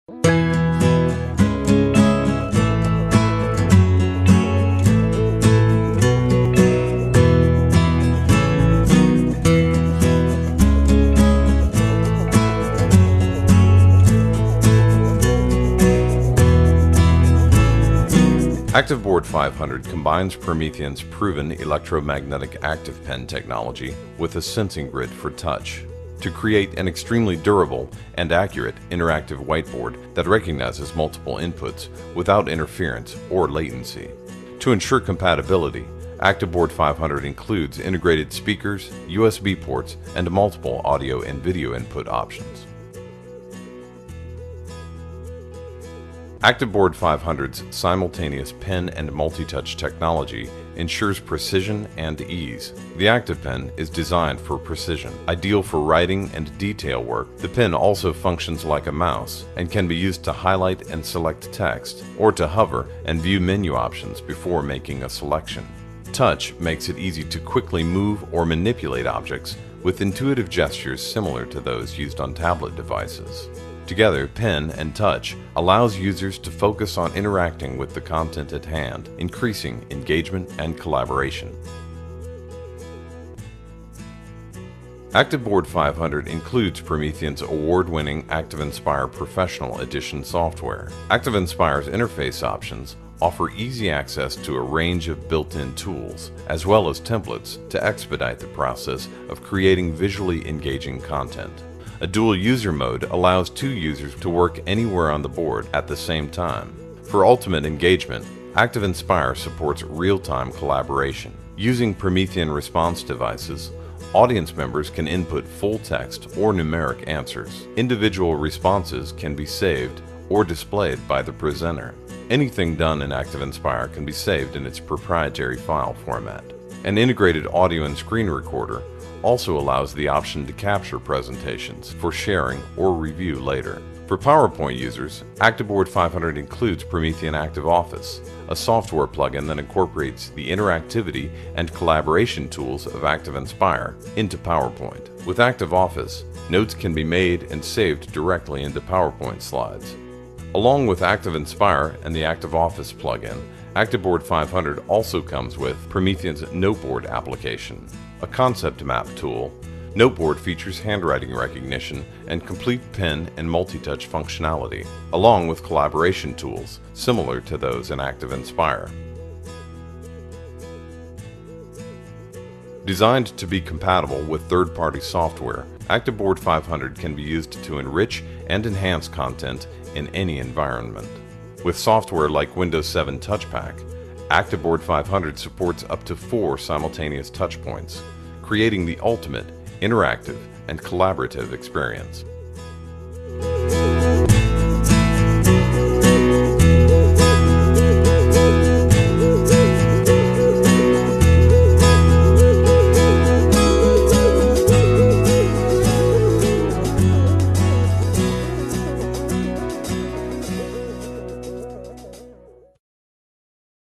Quadro Interactivo ActivBoard 500 da Promethean
Este sistema tecnológico integrado combina as mais recentes descobertas, na área da interactividade em superfície electromagnética e sensível ao multitoque.
Seja no domínio da Educação ou em ambientes empresariais, o ActivBoard 500 torna-se num meio indispensável para qualquer apresentador, formador ou professor.
Forneça aos seus colaboradores a ferramenta mais recente para enriquecer os seus ambientes de aprendizagem, de formação, de colaboração e de partilha de ideias, opiniões, na construção dos seus projectos.